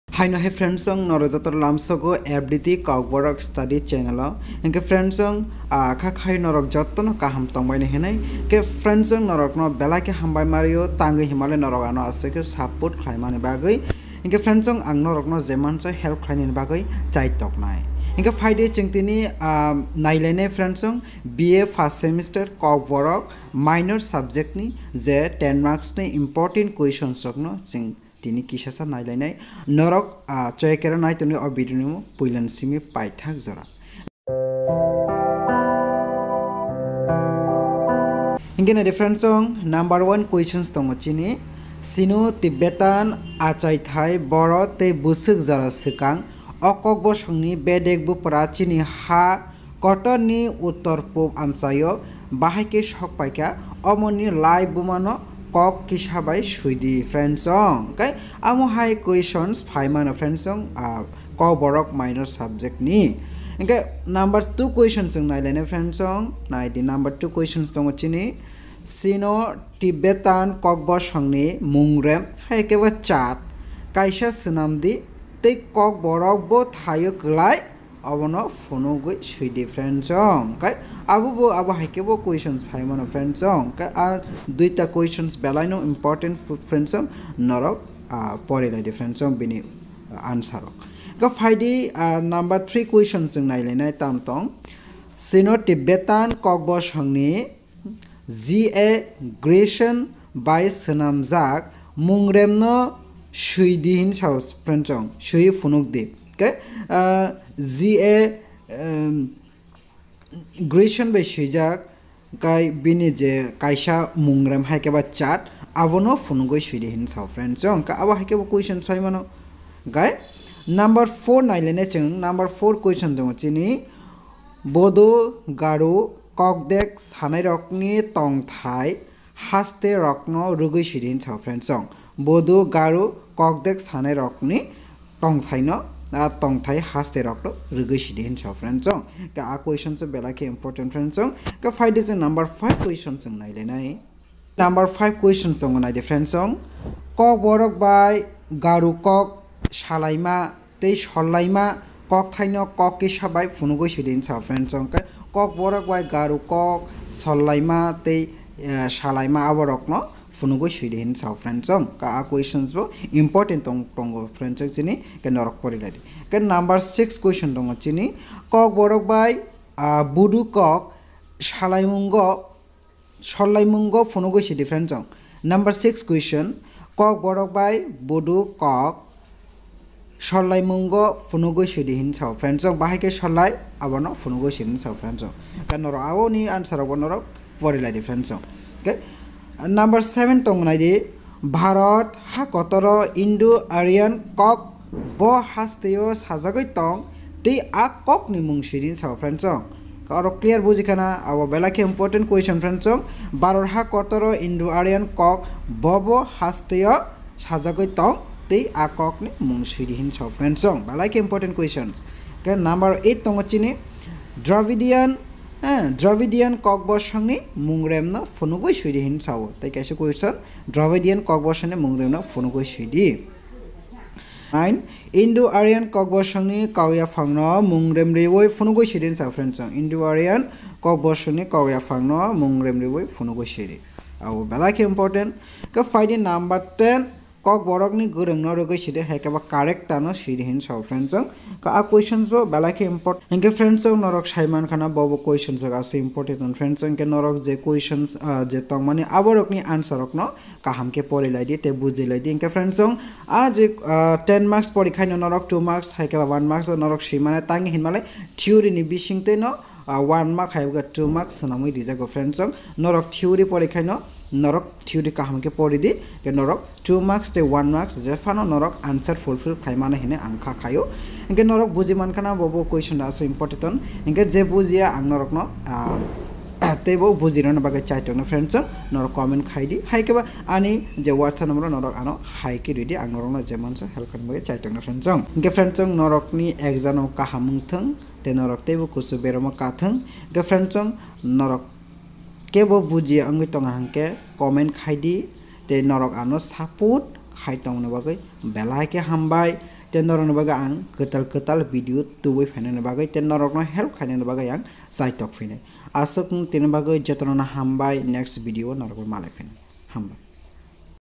B.A 1st Semester || Kokborok Minor || 10 Marks Suggestions -2024 || Tripura University
Kooborok Notes Available 1st Semester Major/Minor/Interdisciplnary